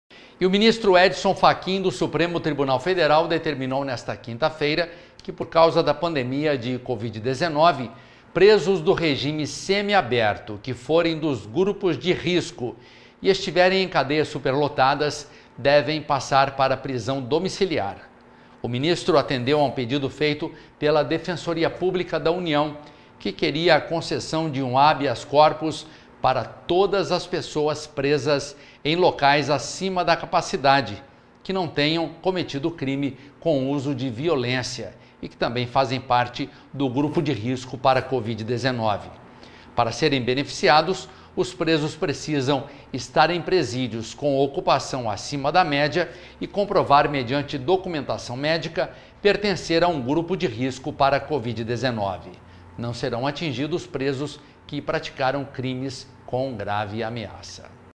Presos podem ser liberados para prisão domiciliar
O ministro Edson Fachin, do supremo tribunal federal, determinou nesta Quinta-Feira que, por causa da pandemia de covid-19, presos do regime semiaberto que forem dos grupos de risco e estiverem em cadeias superlotadas devem passar para a prisão domiciliar.